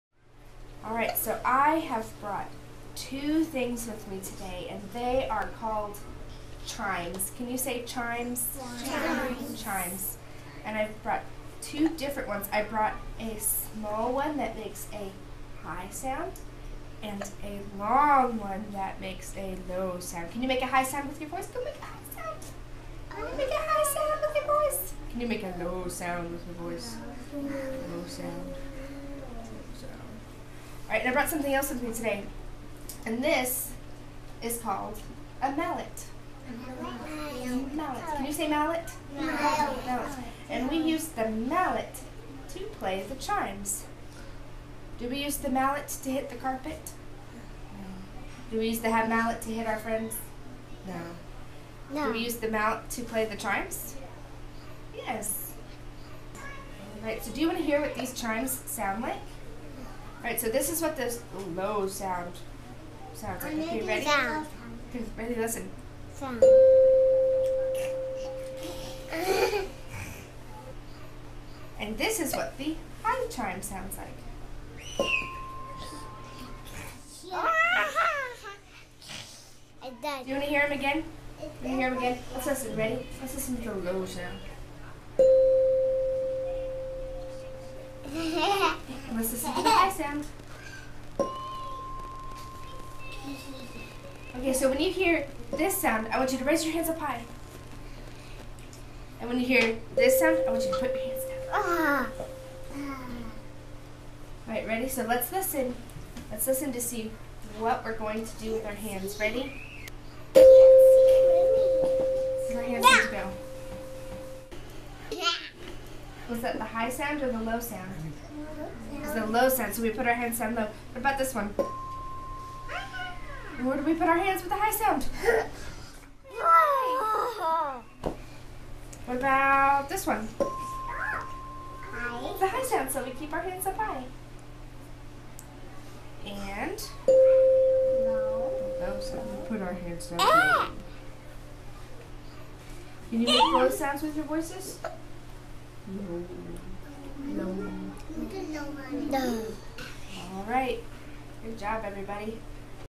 Sounds: High and Low
Alex Taylor  |  Discovery Place Preschool  |  Wichita, KS

This lesson demonstrates how to teach children high and low sounds using chimes and hand motions.